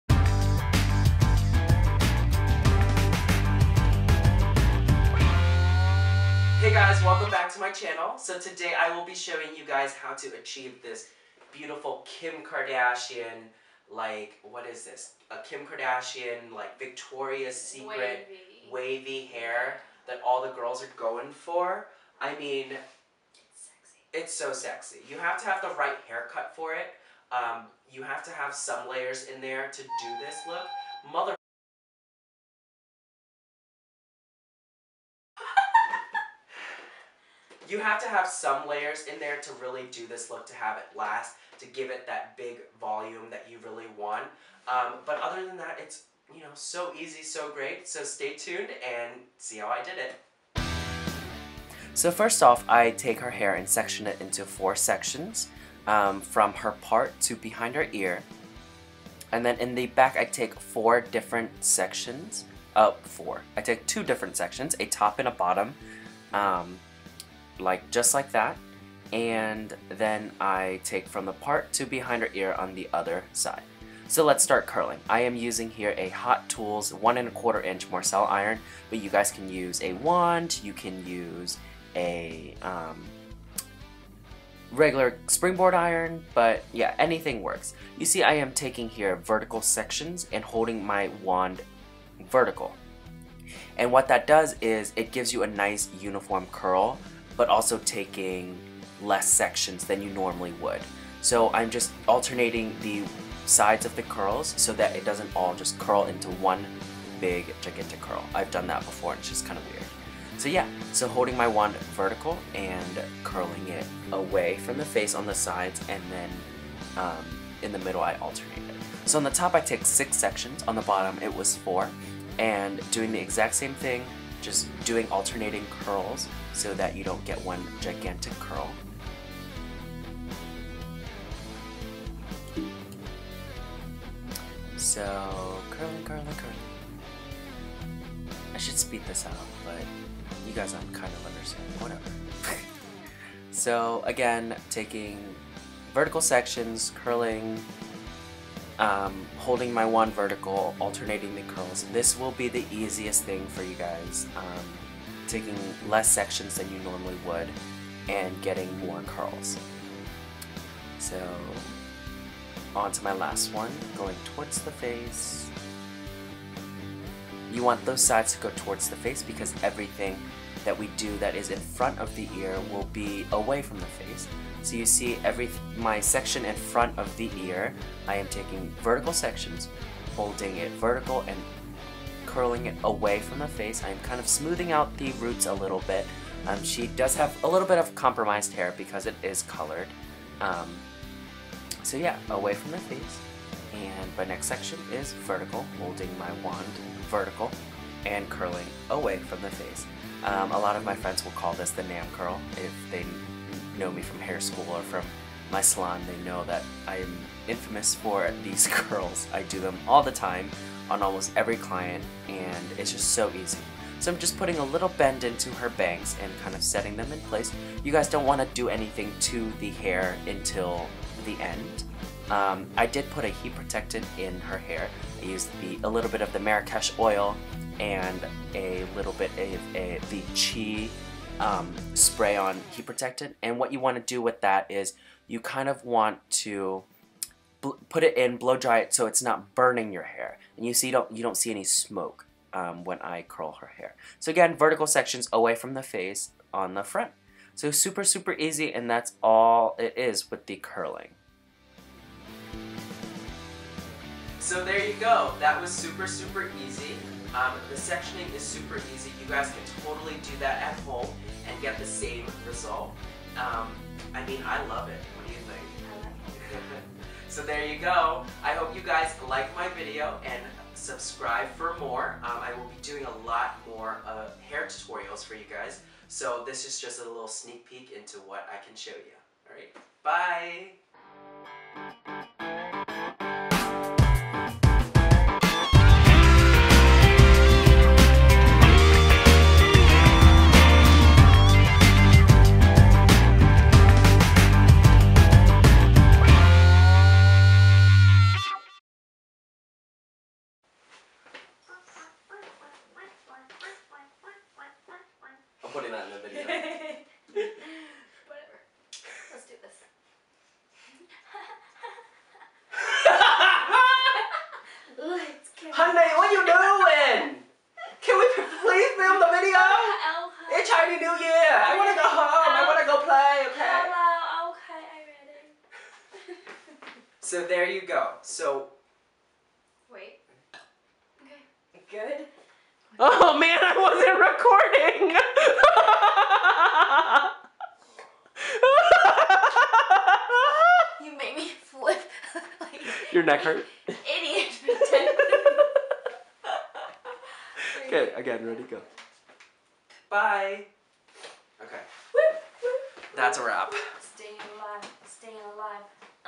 Nam Nguyen - How To: Curl Your Hair Victoria Secret/Kim Kardashian Style
hey guys, In this video I will show you guys how to achieve that kim kardashian /victoria secret hair style

I used the hot tools 1.25 inch curling iron!
as a heat protectant, i used the merrakesh oil and the Chi heat protectant.

dublin forever